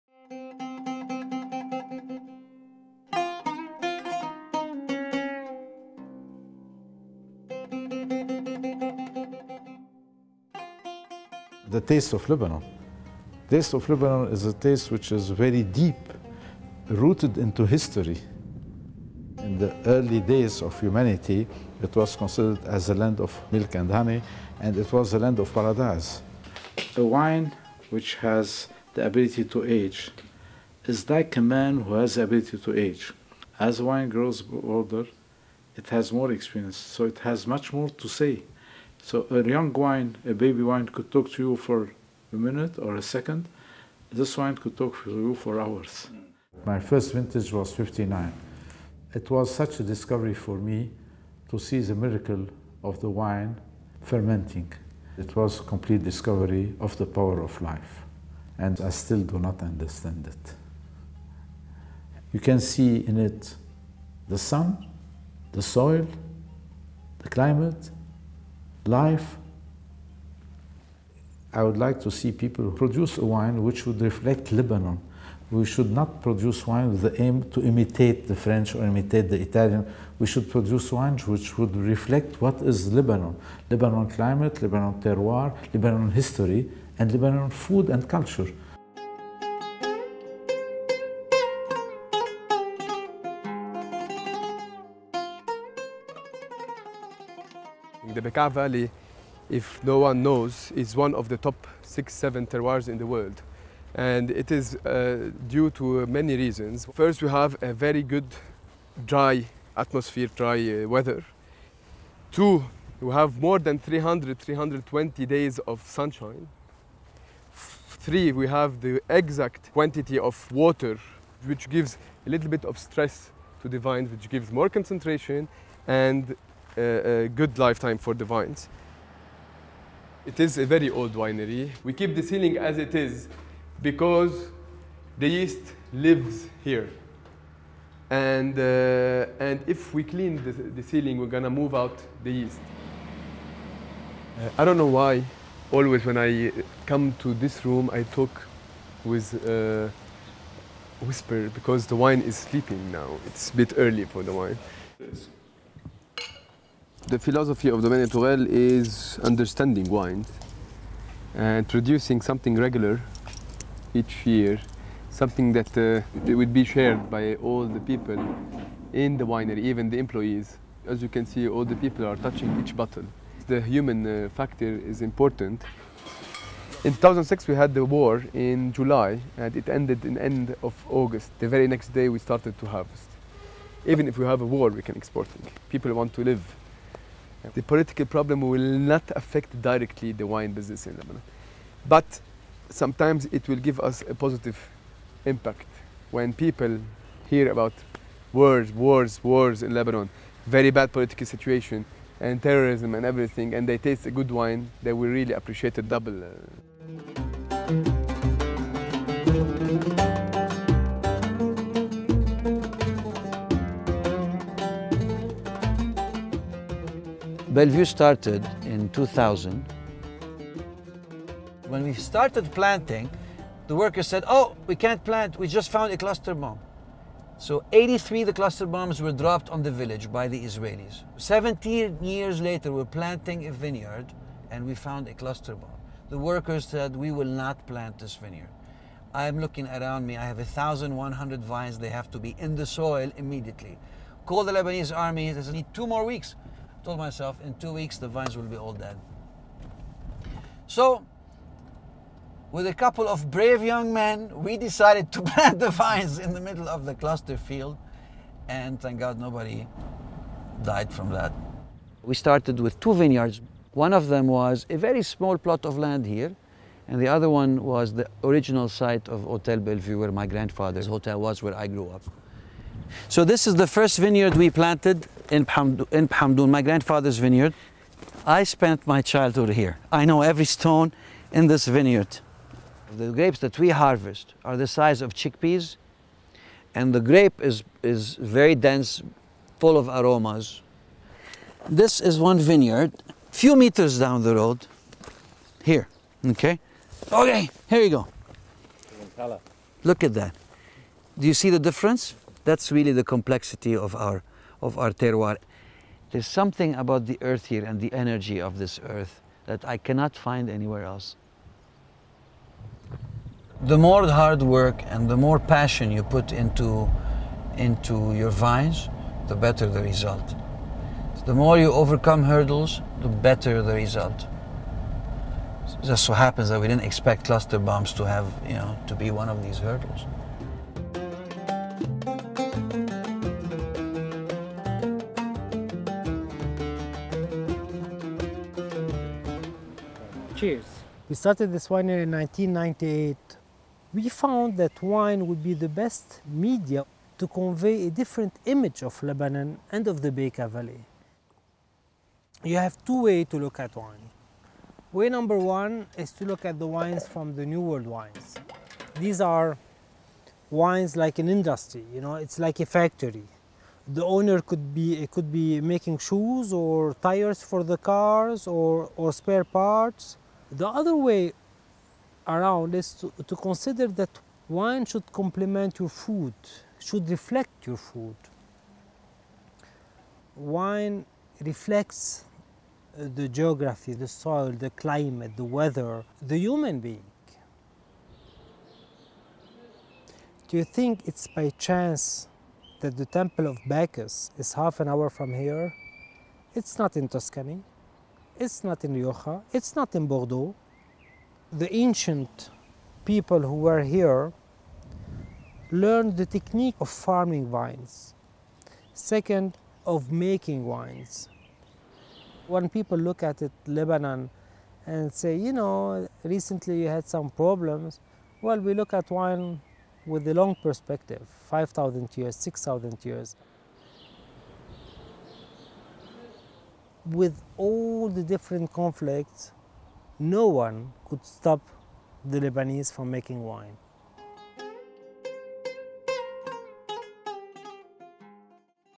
A tour of Lebanon's vineyards
Lebanon's wine-making tradition dates back more than 5,000 years. The vineyards of the Bekaa Valley have survived conflicts and religious divides, but now that the country is enjoying relative peace, its wine industry is flourishing. Many vineyards welcome visitors wanting to discover their history, landscape and people